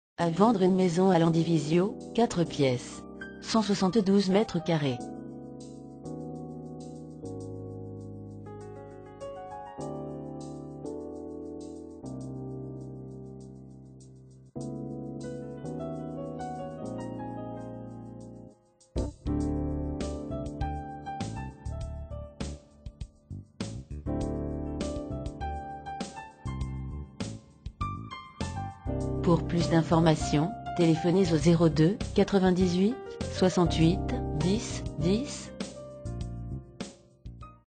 Achat Vente Maison Landivisiau 29400 - 172 m2
Cliquez sur ce lien pour en savoir plus (prix, contacts)

A vendre à Landivisiau une maison de 172 m2.

L agence Cabinet Kerjean Landivisiau vous propose de nombreux autres biens immobiliers.

Annonce immobilière : Achat Vente Maison Landivisiau 29400 Finistère